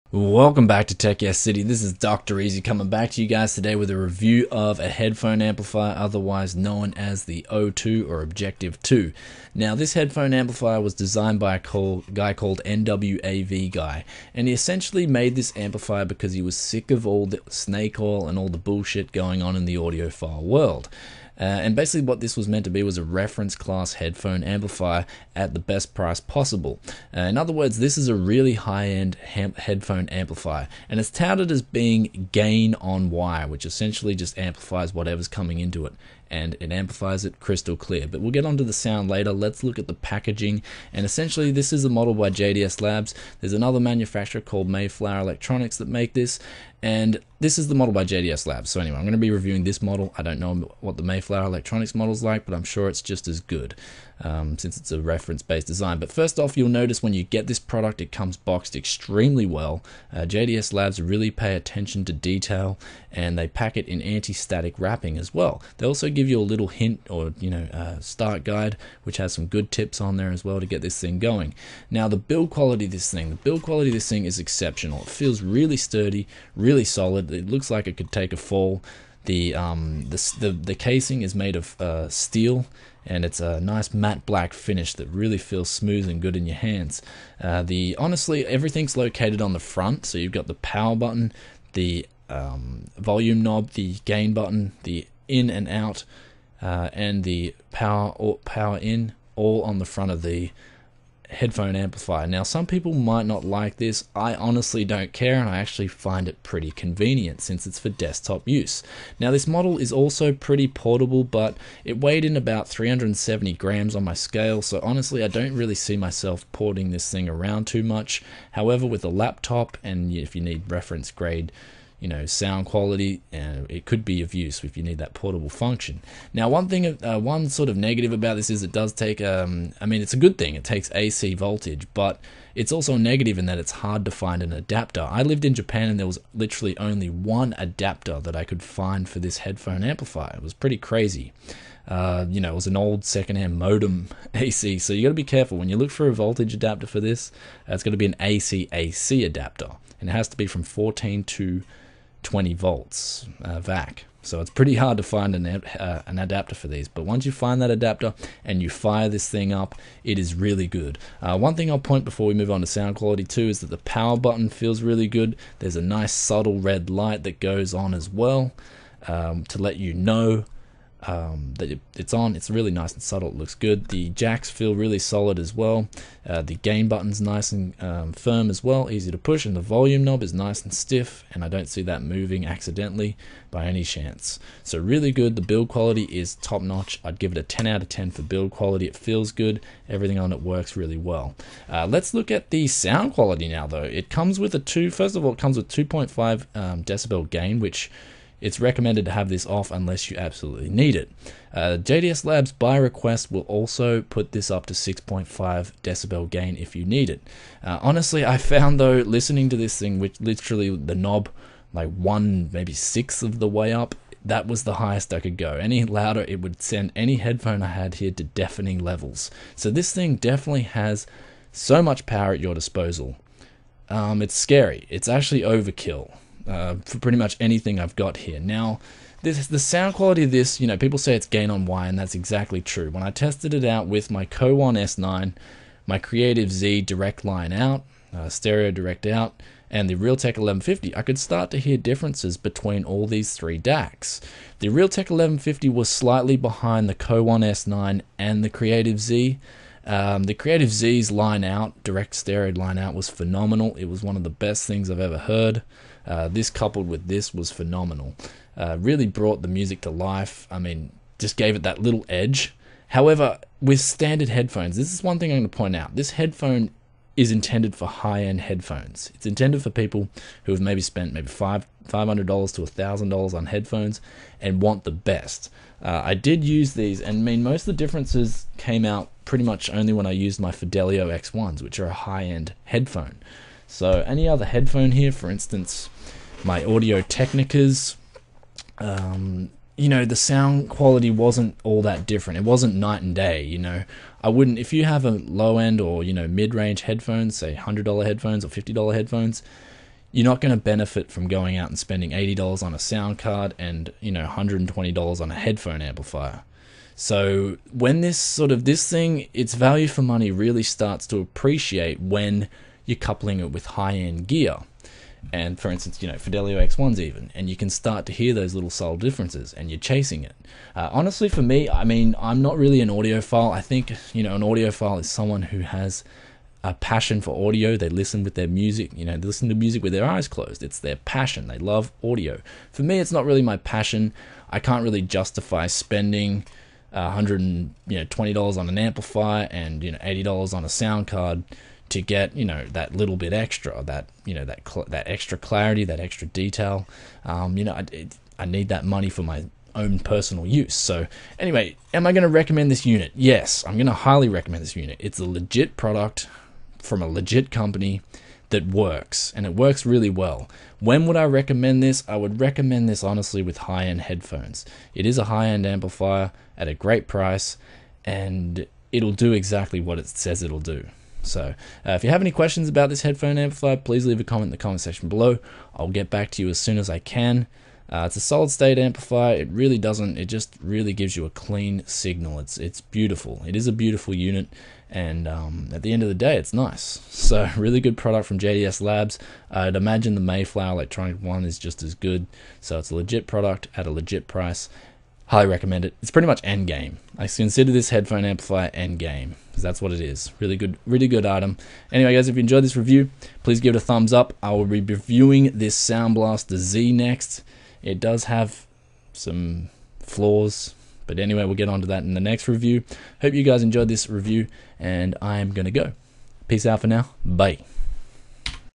JDSLabs Objective 2 Headphone Amplifier Review
Welcome back, this is a review of the JDSLabs Objective 2 amplifier unit.  It is basically one beast headphone amp that will power practically any headphone.  The unit is so clear and provides amazing sound quality that will just to deafening levels - if anything I think this unit is overkill for the average consumer (especially me, I could only have the knob 1/6 of the way up for comfortable listening), though keep in mind with power hungry high impedance headphones, this unit could be a necessity.  Also one more thing to note was that the AC-AC adapter was a bit of a pain to get, so keep that in mind if you are buying internationally.

Here are the specs:
Frequency Response +/-0.01 dB (20Hz-20kHz)
THD 1 Khz 150 Ohms 0.0016%
IMD CCIF 15 Ohms 0.001%
IMD SMPTE 0.002%
Noise (ref 400 mV) -105 dB
Max Output (33 Ohms) 613 mW
Max Output (150 Ohms) 355 mW
Max Output (600 Ohms) 88 mW
Output Impedance 0.54 ohms
Crosstalk (15 ohms) -65 dB
Channel Balance (50% volume) 0.6 dB
Battery Run Time 6-8 Hours